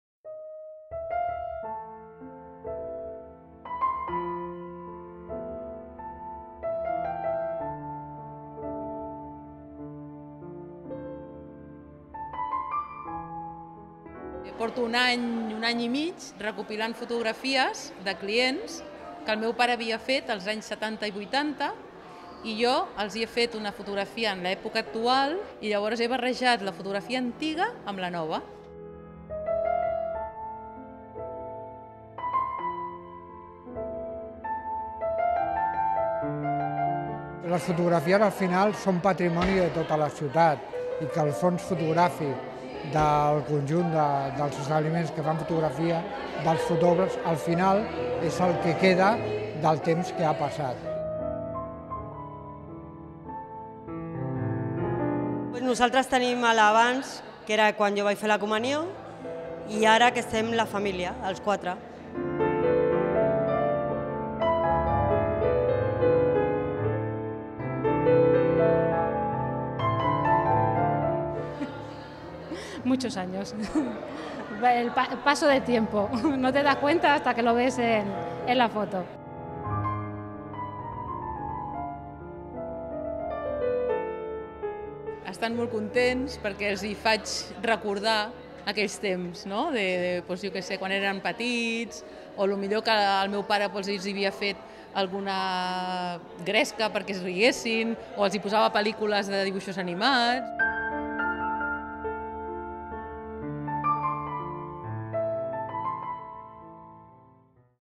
La història familiar de Viladecans
Fotografia Raventós celebra els seus 40 anys amb una exposició a la Biblioteca de Viladecans on combina antigues instantànies familiars amb les seves contrapartides actuals. L’exposició es pot visitar fins al 20 de setembre.